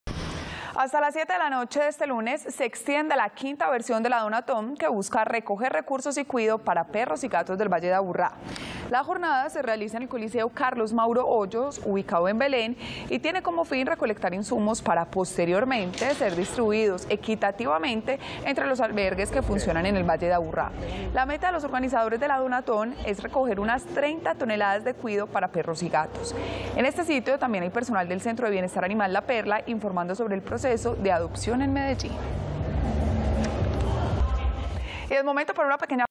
La quinta versión de la Donatón terminará hoy a las 7:00 p.m. [Noticias] - Telemedellín
Hasta las 7 de la noche de este lunes 19 de agosto se extiende la quinta versión de la Donatón que busca recoger recursos y cuido para perros y gatos del Valle de Aburrá.